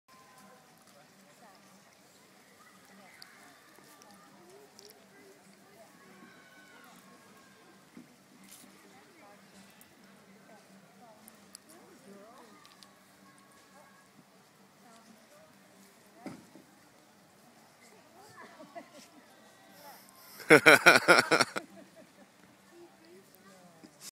Thai chi at wheatland music festival
My aunts doin Their thing!!!